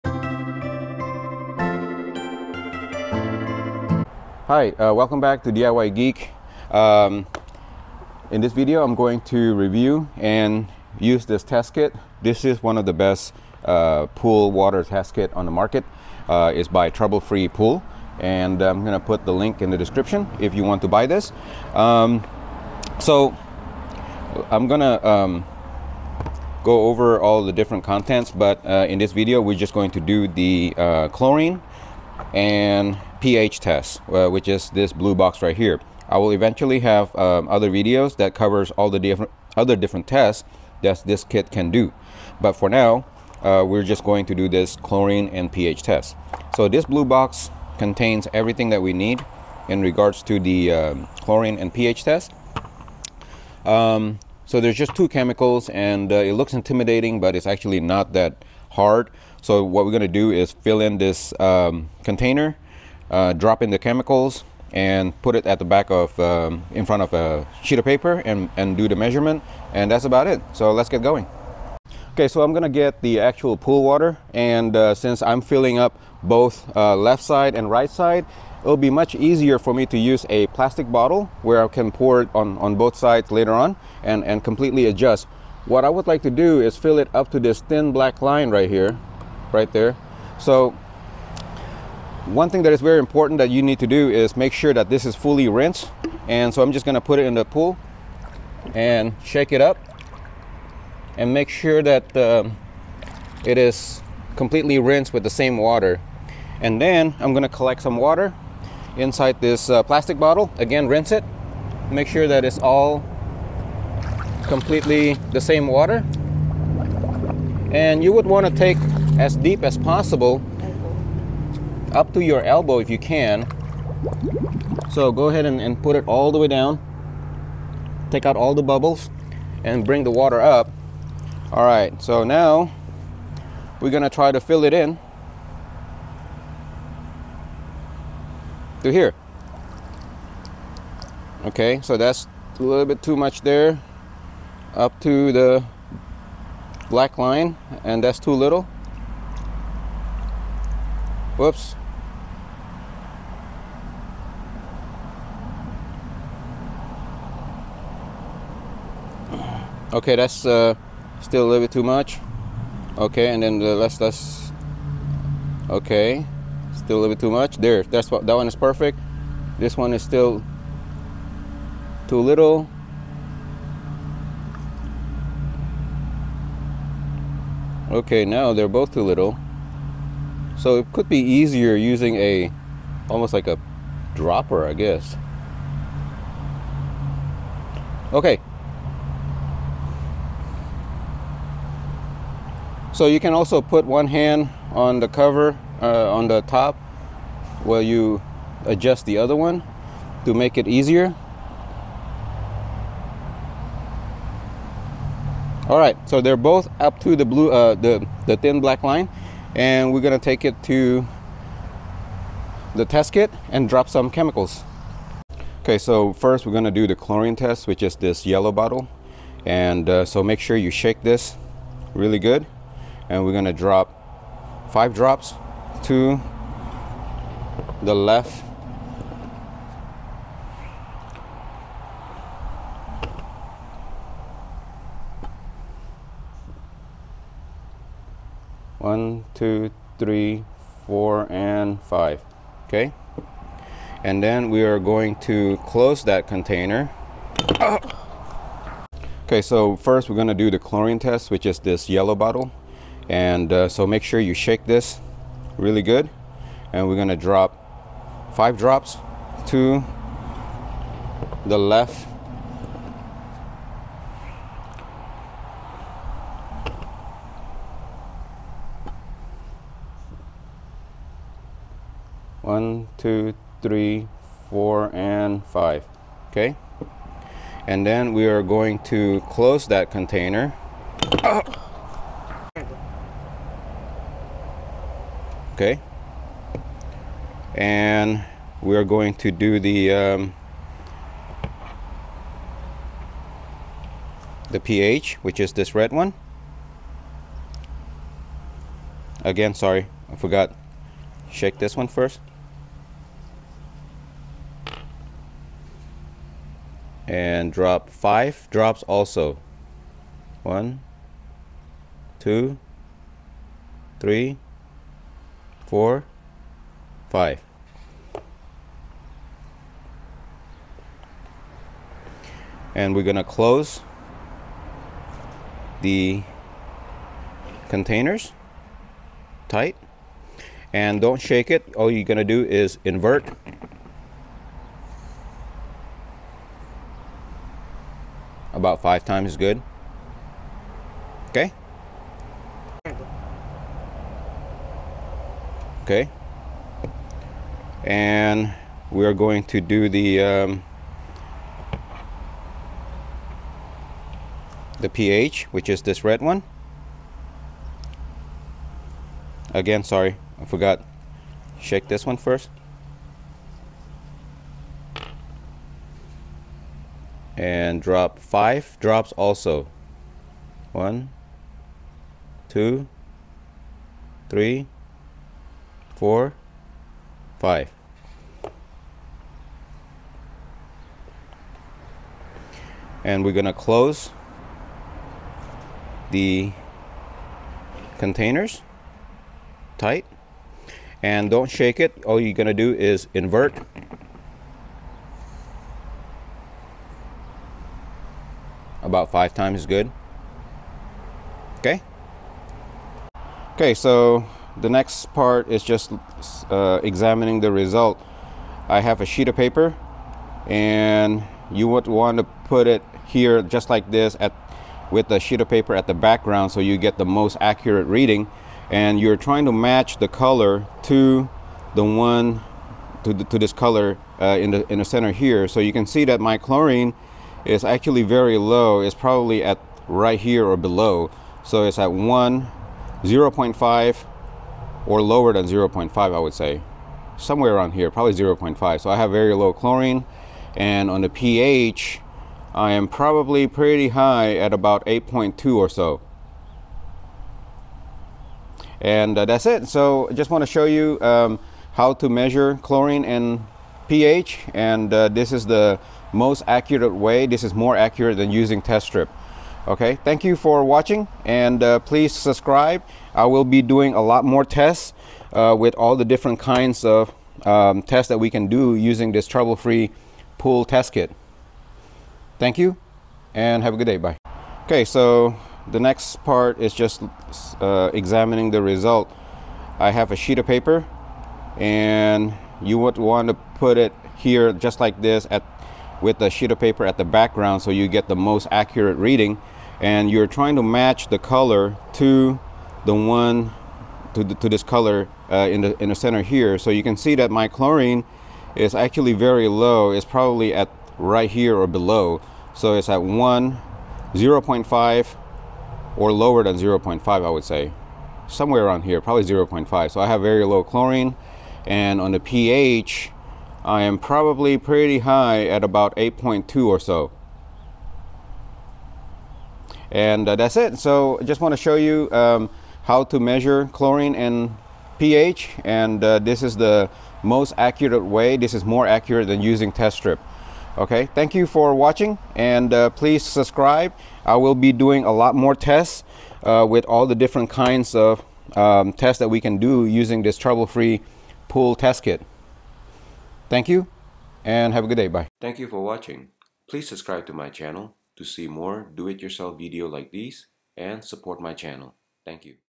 POOL WATER TEST KIT - TESTING CHLORINE AND PH LEVEL
I have a Pool Water Test Kit from Trouble Free Pool (TF Pool Test Kit).
I am showing how to test for Chlorine and pH levels.